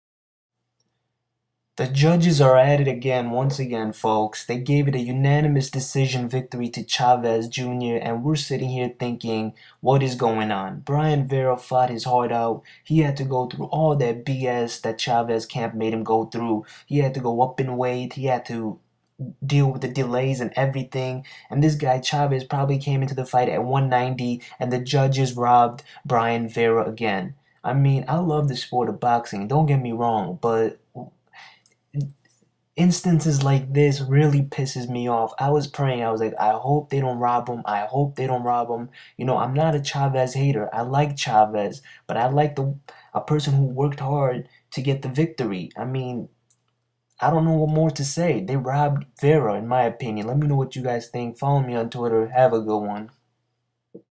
BOXING TALK: ROBBERY: JULIO CESAR CHAVEZ JR DEFEATS BRIAN VERA: MY THOUGHTS & REACTIONS
my thoughts and reactions to chavez jr vs brian vera!